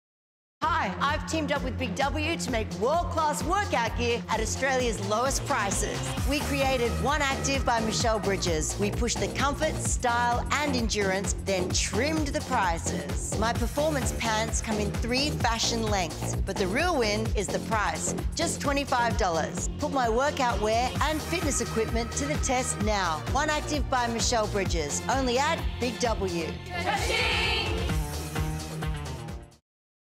BIG W ONE ACTIVE Michelle Bridges
Check out Michelle Bridges' latest apparel range ONEActive, available exclusively from Big W. This video appears in issue 4 of OH! Magazine